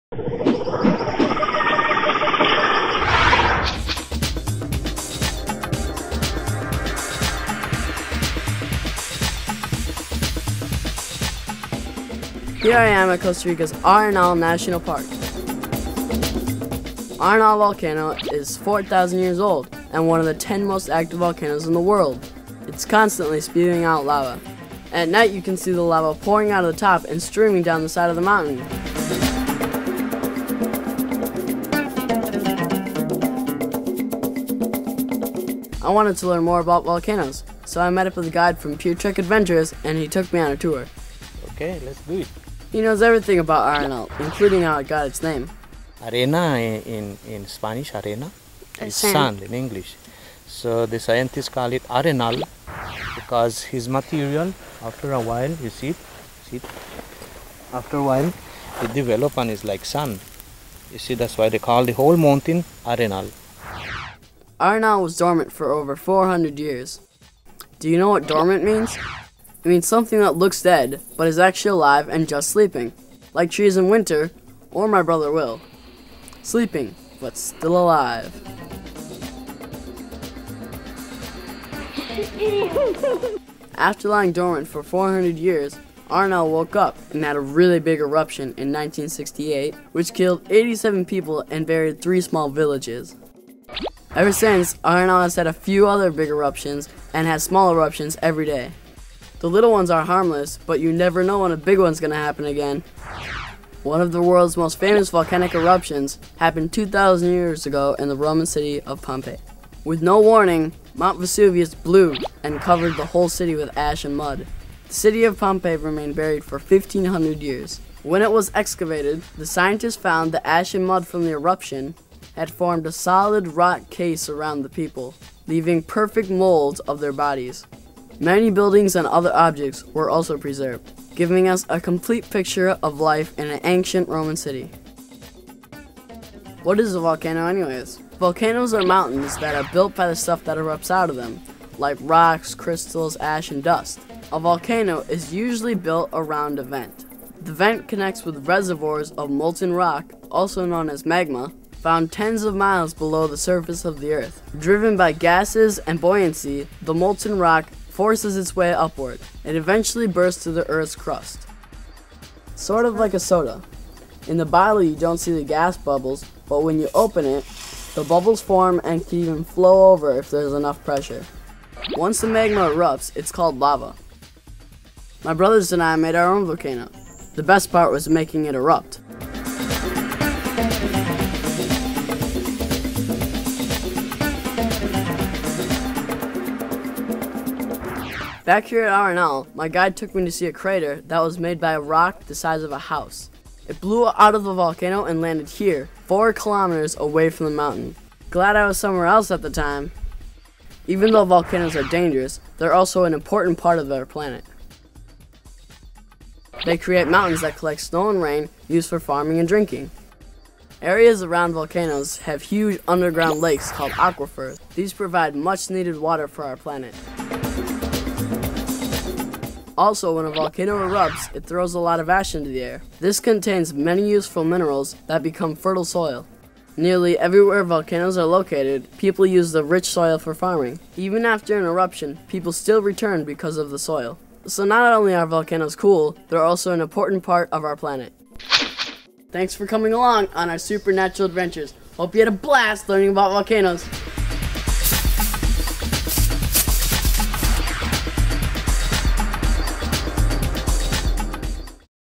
Arenal Volcano National Park
A tour of Arenal Volcano in Costa Rica. Science report on how volcanoes work and interact with the rest of the planet. A Super Natural Adventure up close at this active hot spot.